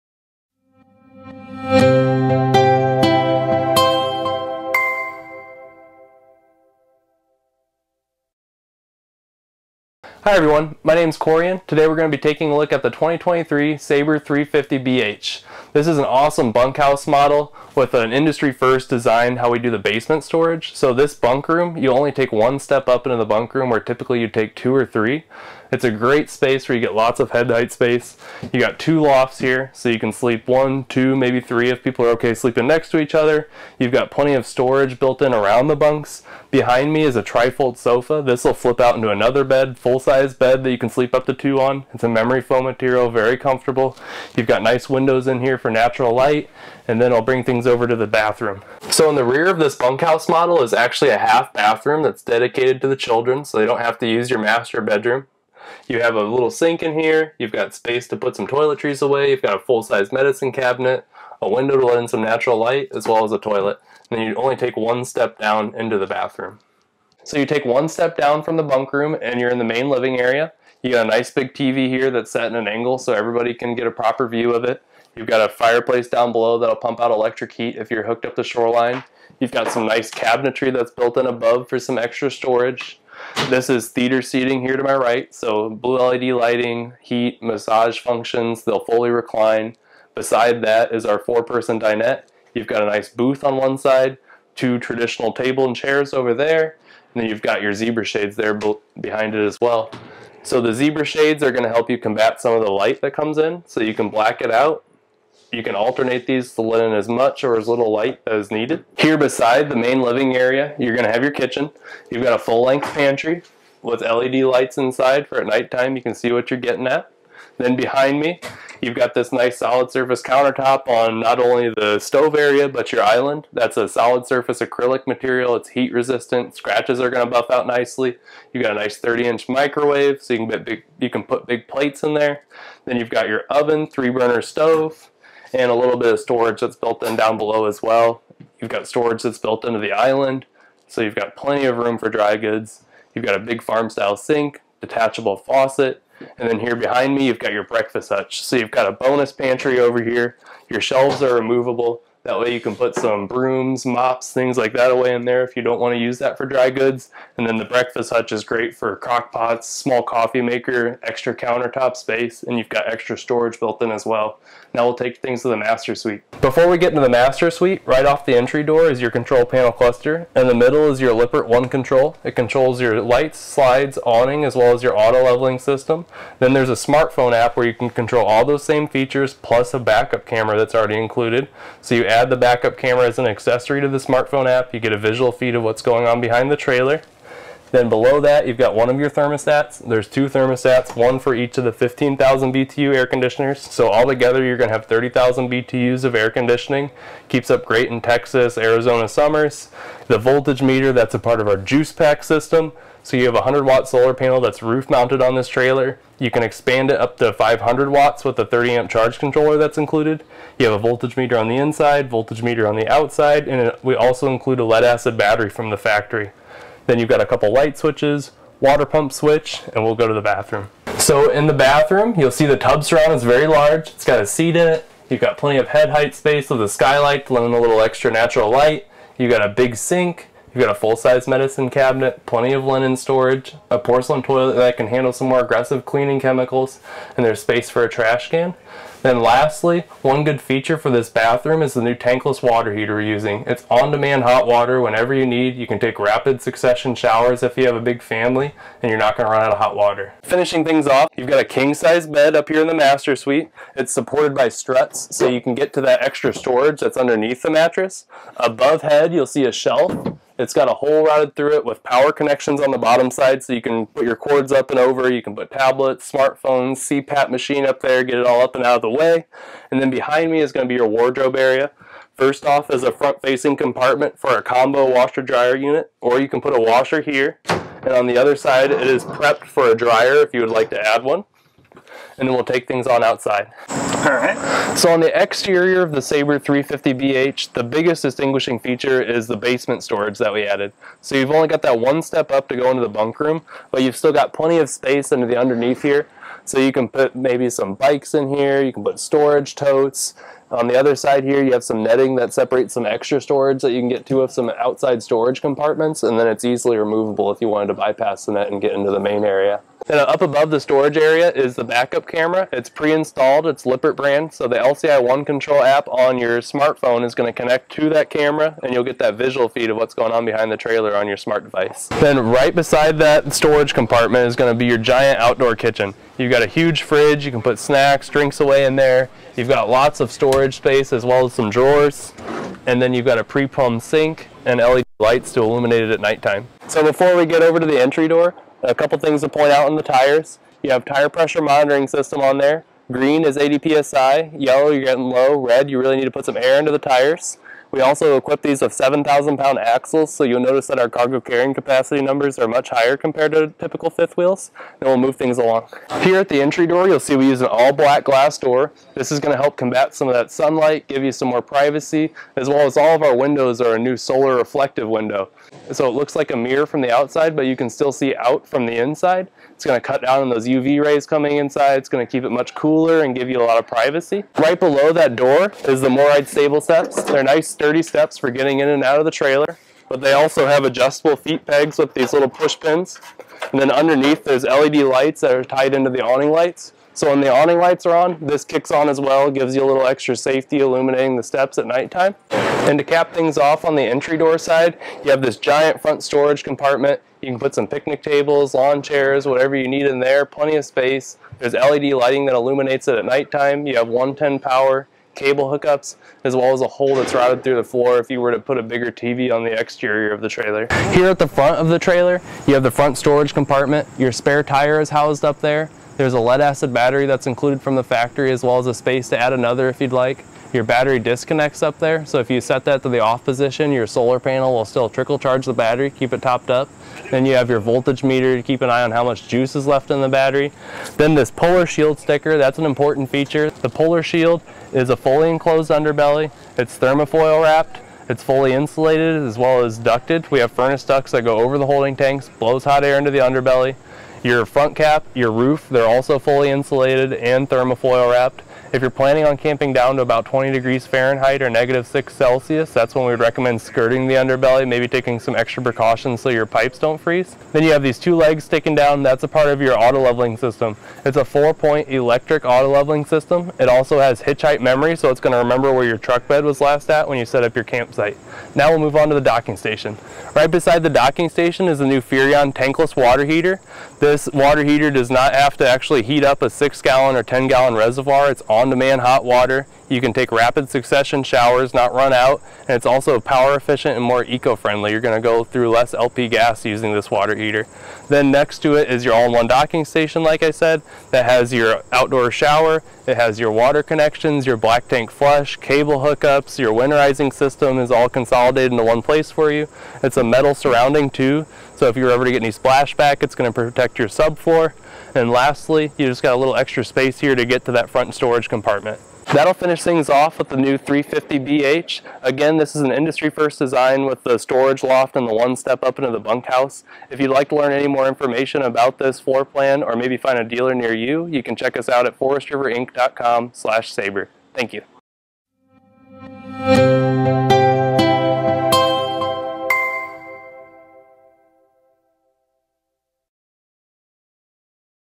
Tour the 2023 Sabre 350BH
Meet the all new 2023 Sabre 350BH!  The rear private bunkhouse includes bunk beds, a tri-fold sofa, a half bath, plus basement storage below for luggage, coolers, or yard games. You'll enjoy your own space up front in the private bedroom that includes a king bed slide out, a shelf for a CPAP machine, plus a front wardrobe with washer and dryer prep if you choose to go full-time. The main living and kitchen area will be a favorite place to meet together since there is an LED TV, a fireplace, plus a theater seat. The chef will love the extra counter space provided by the kitchen island, and there is a ceiling fan here to cool them off when they're cooking.